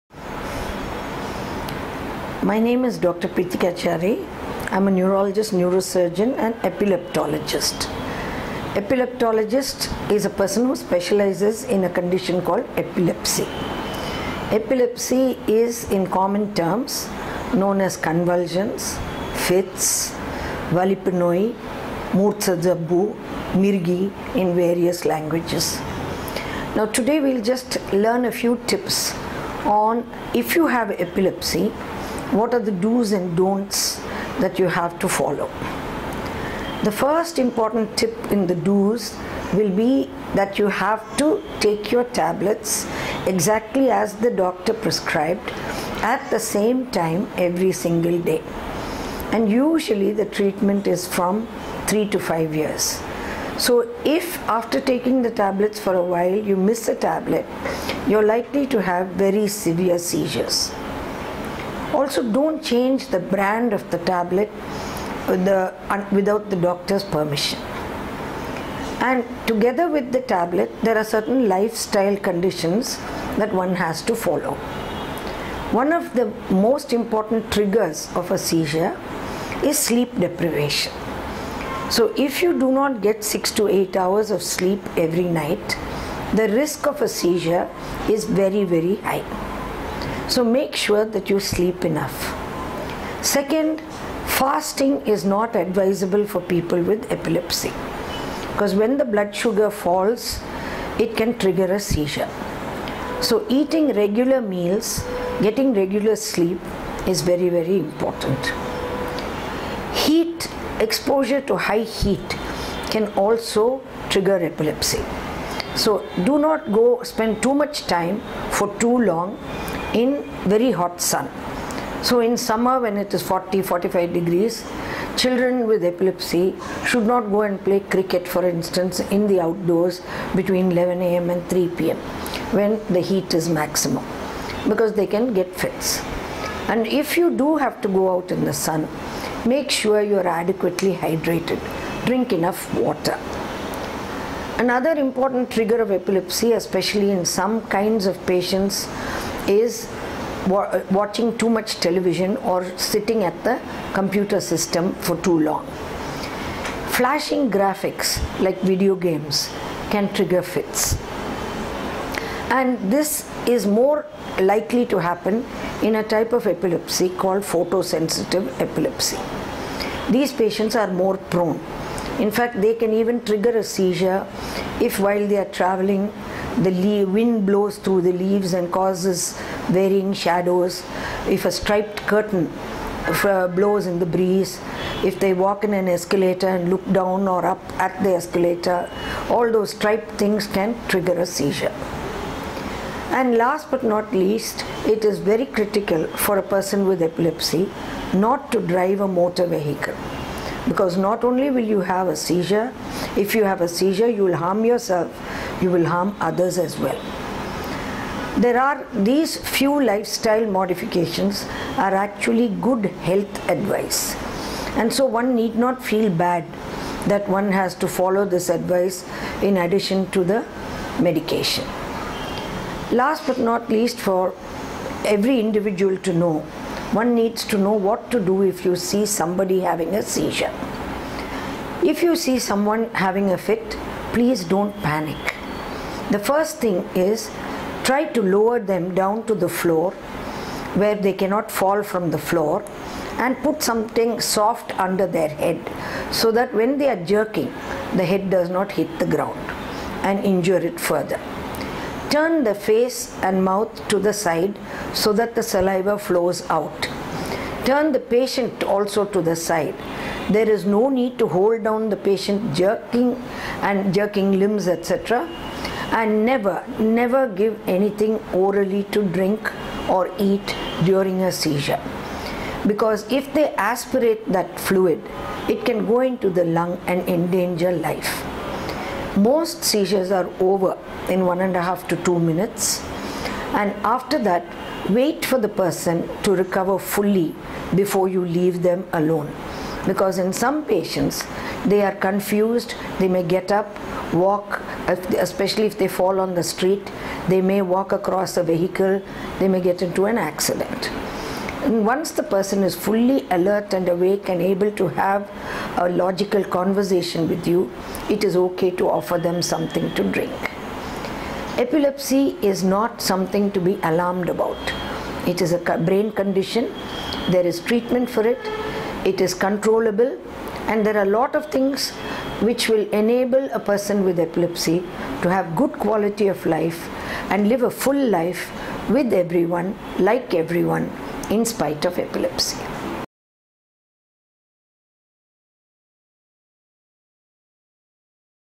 How can you prevent a seizure? | வலிப்புத்தாக்கத்தை எவ்வாறு தடுப்பது?
Triggers for seizures
• Sleep deprivation
• Fasting
• Exposure to high temperatures
• Watching too much television
• Flashing graphics
• Missing medications

Tips for seizure prevention

Correct and on-time use of medication, regular sleep, and a healthy diet are crucial to managing epilepsy. The patient must also stay hydrated, stay away from extreme temperatures, and reduce exposure to flashing lights to prevent epileptic episodes.

First aid for seizures

If you are in the vicinity when someone experiences a seizure, do not panic. Lower the patient to the floor and turn them on their side. Wait until they are fully conscious before you offer water.

Watch Dr. Pritika Chary of Kauvery Hospital, one of the best neurosurgeons in Chennai, explain the simple lifestyle dos and don’ts to consider while managing epilepsy, commonly known as fits.

𝐏𝐨𝐝𝐛𝐞𝐚𝐧